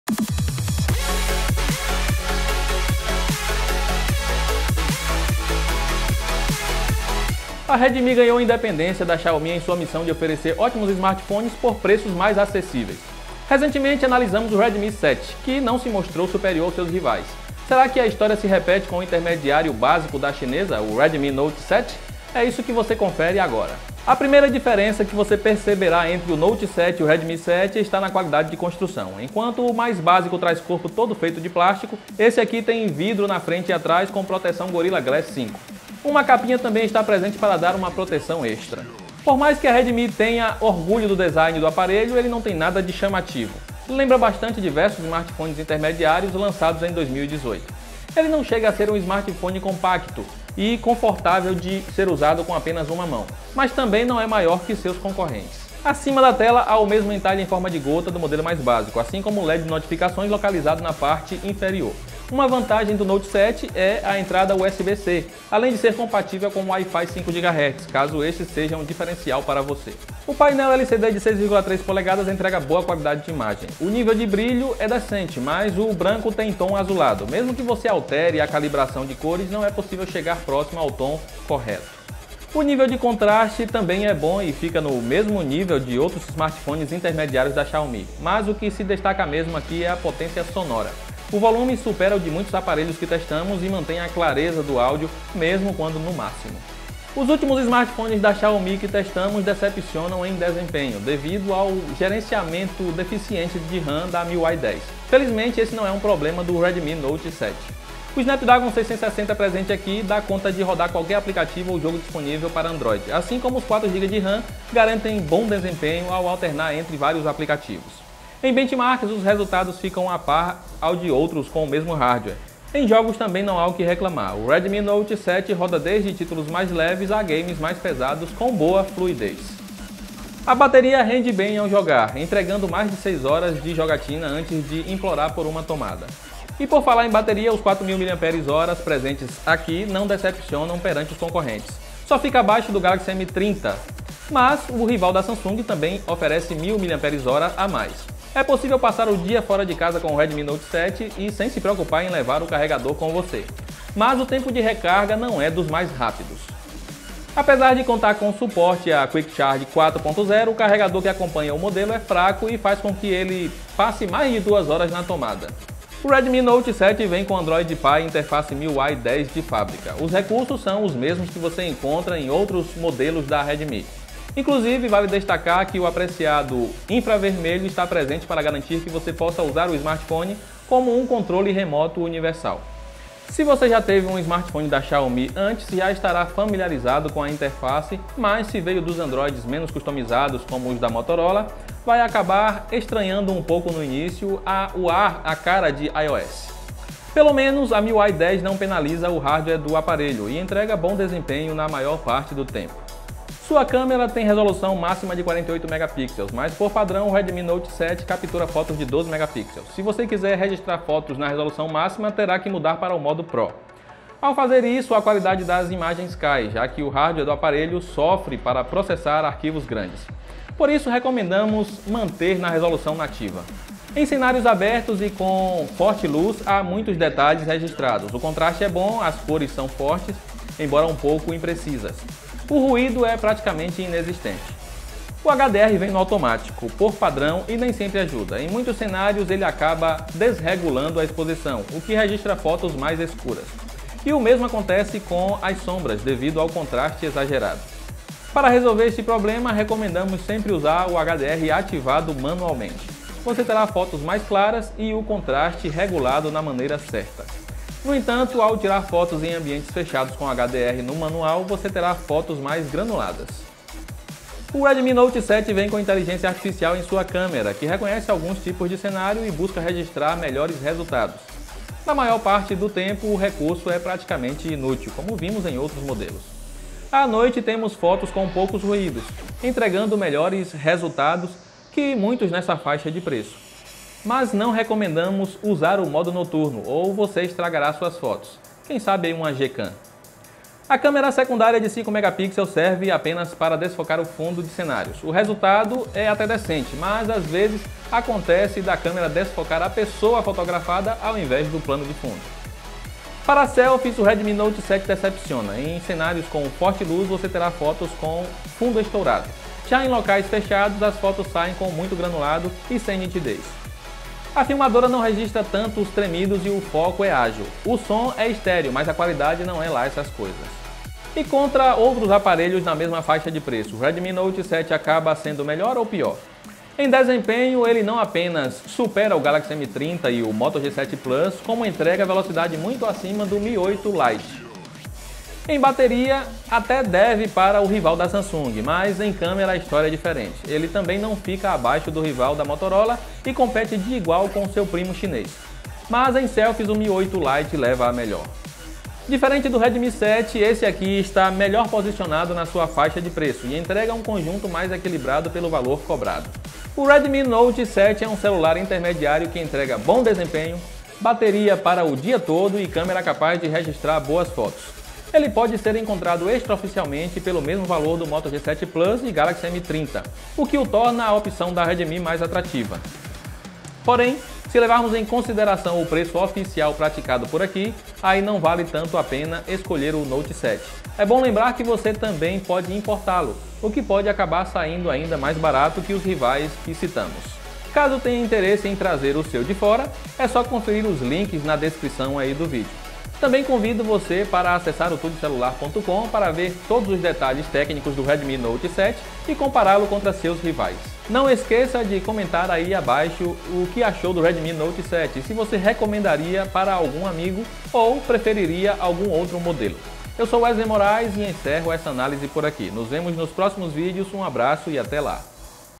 Redmi Note 7 inaugura a categoria "matador de intermediários" | Análise / Review
O intermediário Redmi Note 7 da Xiaomi é o novo queridinho daqueles que buscam um aparelho equilibrado e com preço abaixo de dois salários mínimos. E ele mostrou que faz jus à sua popularidade.